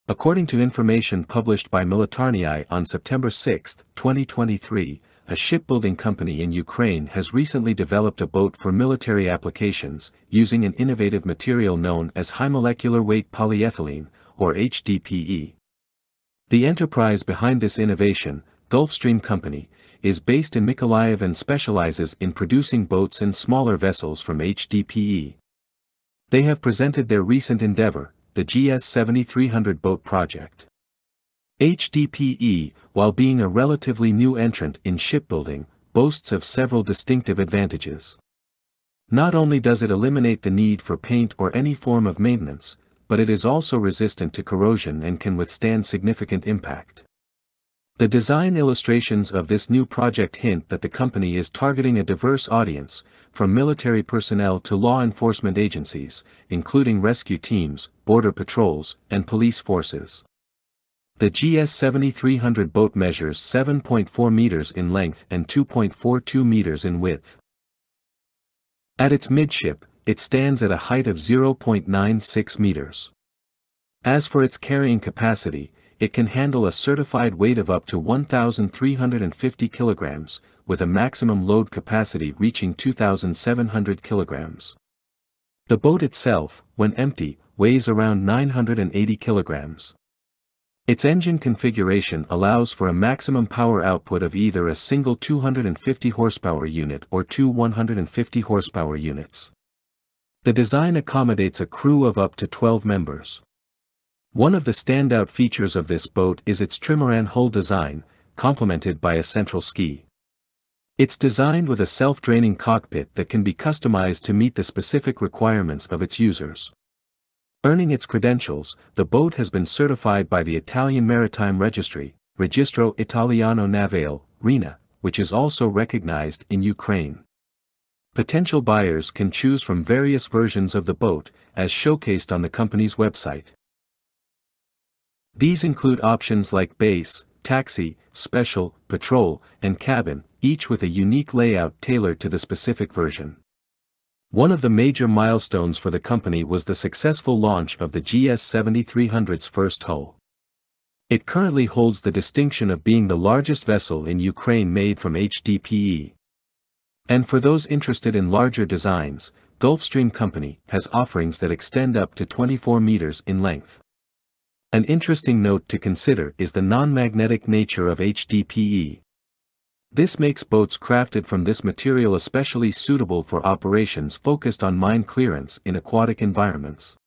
Ukrainian Gulfstream Company unveils GS7300 fast boat
According to information published by Militarnyi on September 6, 2023, a shipbuilding company in Ukraine has recently developed a boat for military applications, using an innovative material known as high-molecular-weight polyethylene, or HDPE.